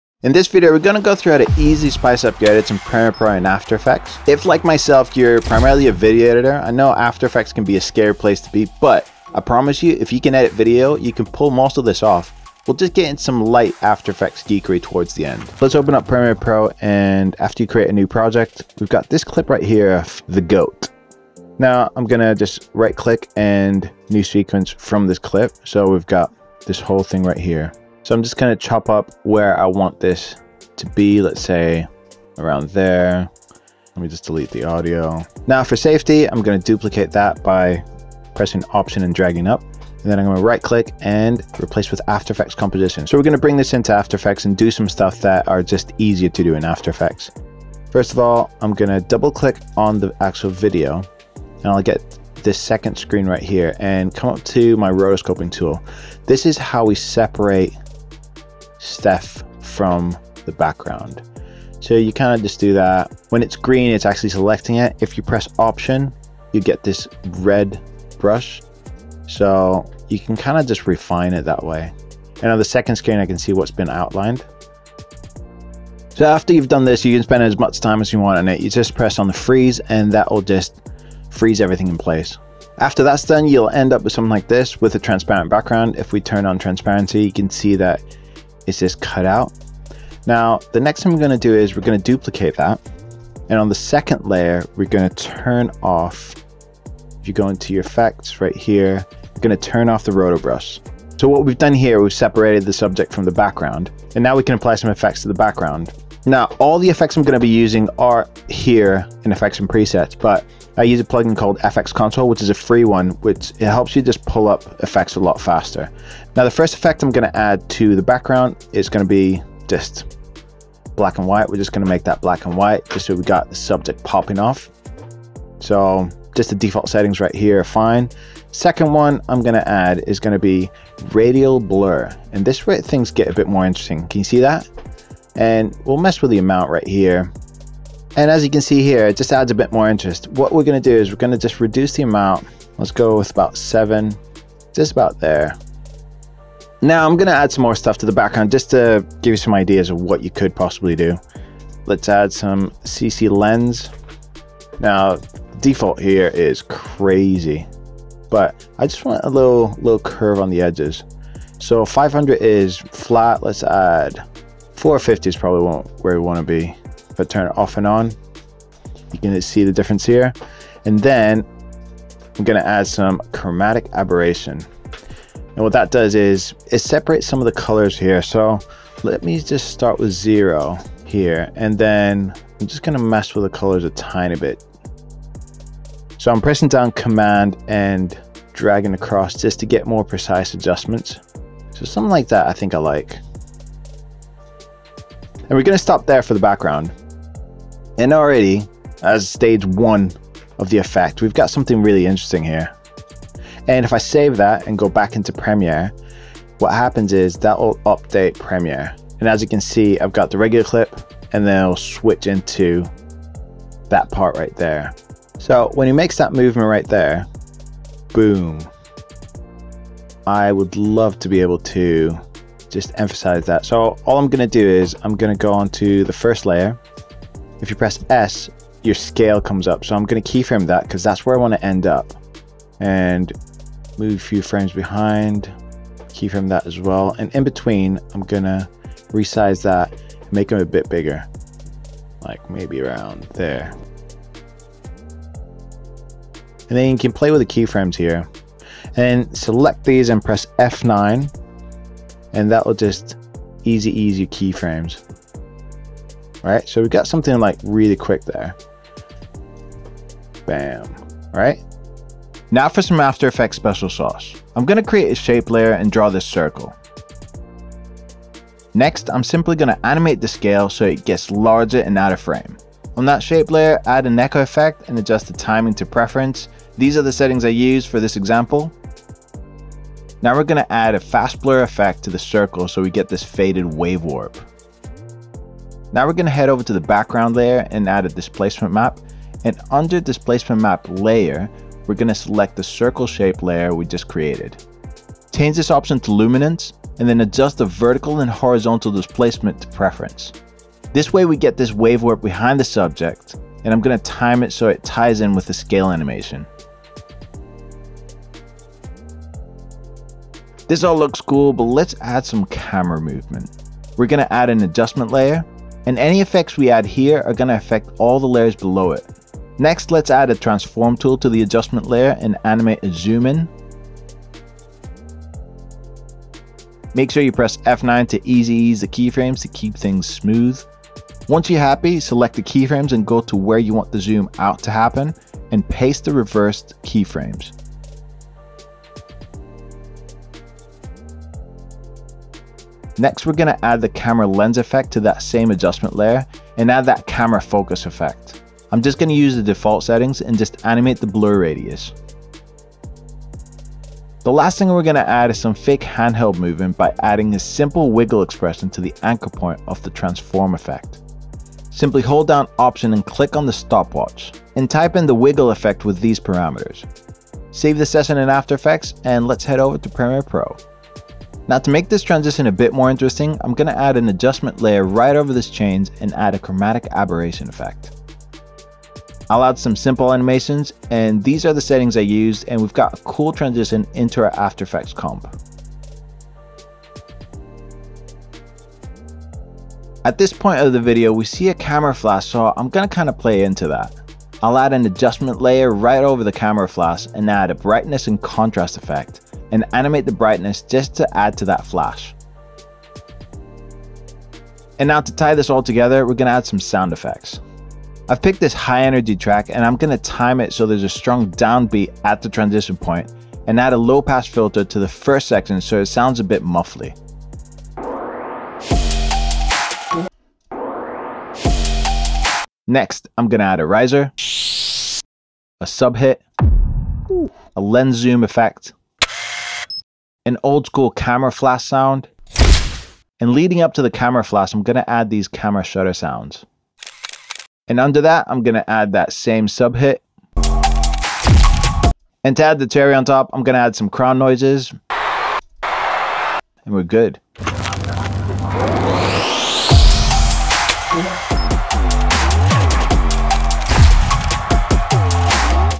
Sports VFX you SHOULD use | After Effects | Premiere Pro
In this video Dave shows us in After Effects and Premiere Pro how to do a quick sports vfx effect to easily add more interest and energy to your video edits.

*GEAR*
🎥 Canon R7
🔍 Sigma 18-35mm
🔍 Canon EF-S 17-55mm f/2.8 IS
🔦 Zhiyun Molus X100
🔦 INKEE Inflatable tube light 12W
🎙️ Universal Audio SD-1
🎙️ Mic Stand